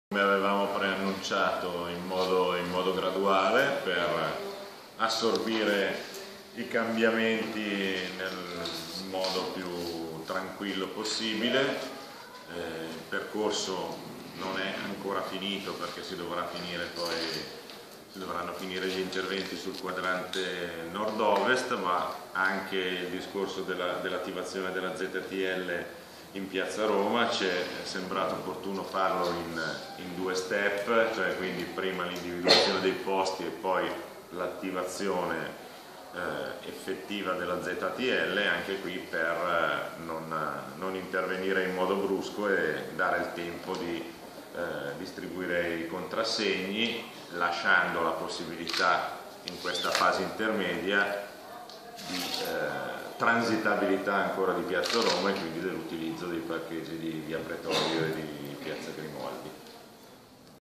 IL SINDACO LUCINI:"SARA' UNA PARTENZA GRADUALE PER LA ZTL"
Parla il sindaco di Como, Lucini: lo stato di avanzamento dei lavori per la nuova Ztl del centro città